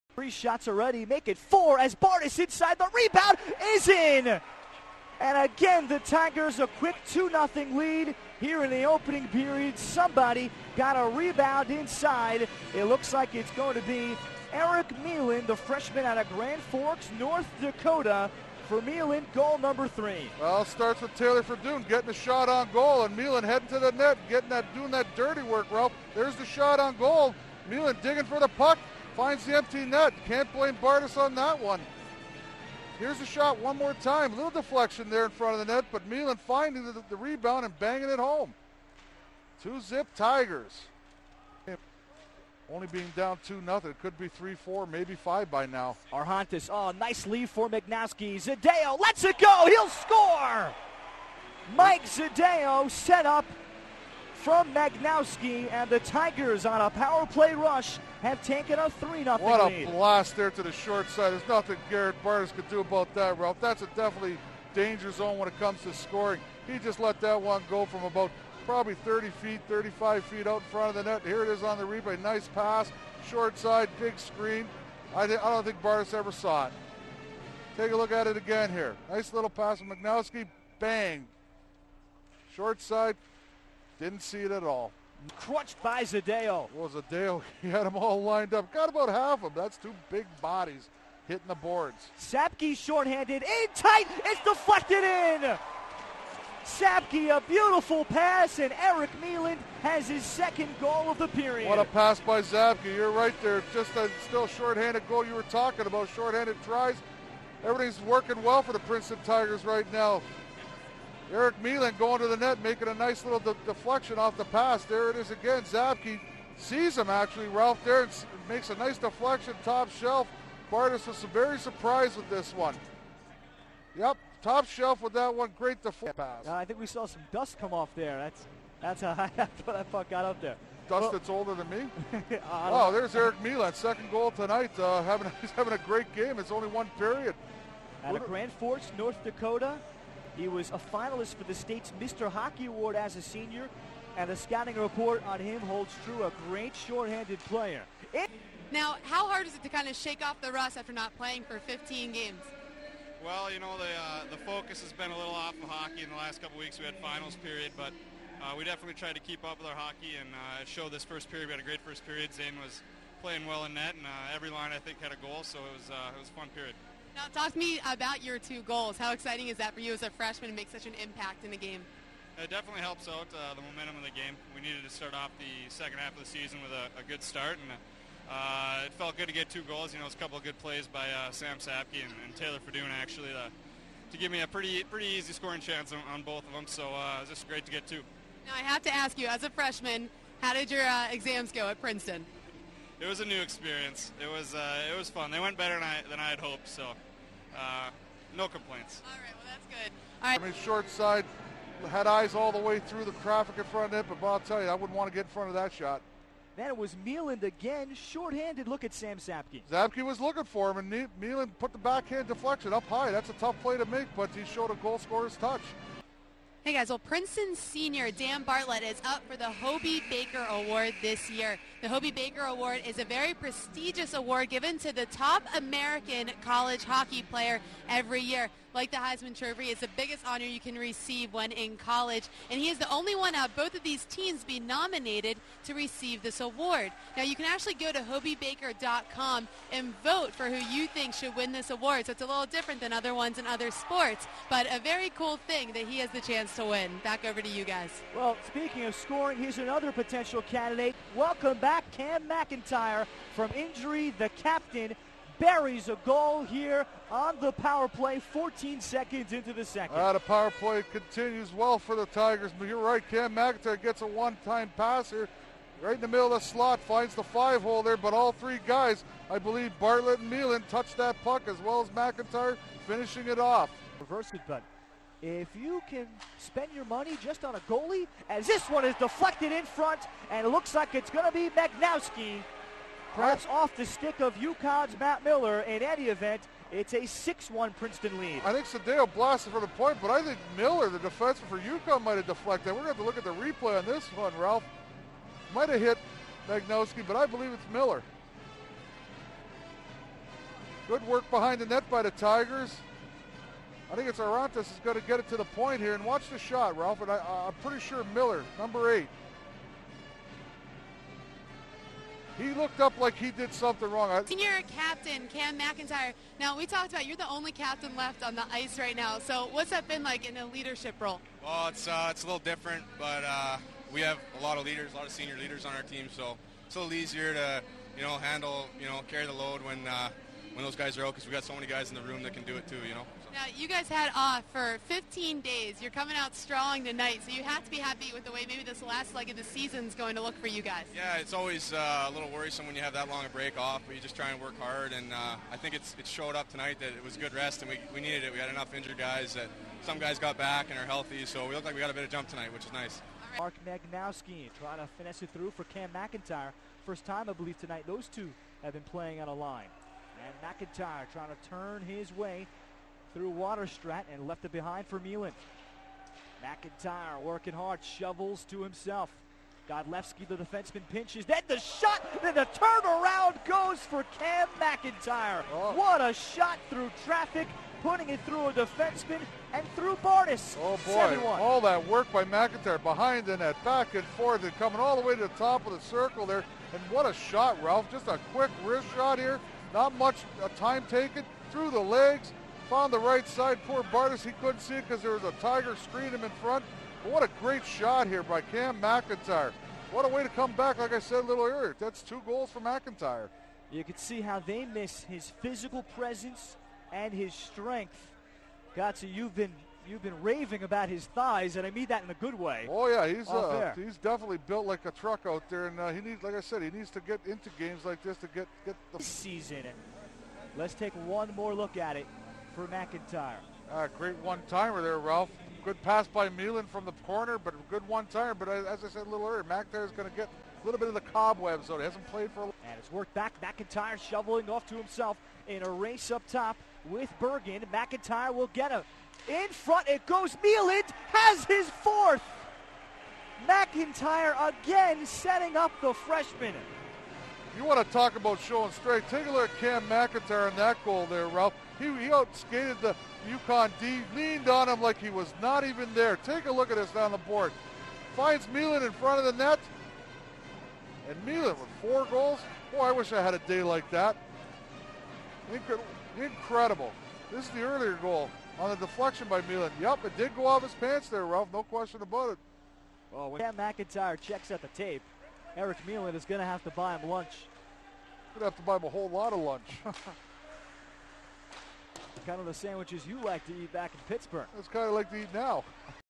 Princeton Goals UConn 8-1 1-25-10
1 1st 06:14 EV Magnowski, Mark/4 Kramer, Mike/8
2 1st 08:16 PP Meland, Eric/3 Fedun, Taylor/7  Bartlett, Dan/7
3 1st 13:16 PP Sdao, Michael/3 Magnowski, Mark/ Arhontas, Matt/5
4 1st 17:28 SH Meland, Eric/4 Sabky, Sam/  Schroeder, Brad/4
5 2nd 00:14 PP MacIntyre, Cam/5 Meland, Eric/ Pallis, Derrick/4
7 2nd 19:39 EV Sdao, Michael/3  Arhontas, Matt/6    Kramer, Mike/9
8 3rd 01:10 EV MacIntyre, Cam/6 Meland, Eric/ Magnowski, Mark/9
9 3rd 04:59 EV Meland, Eric/6  MacIntyre, Cam/4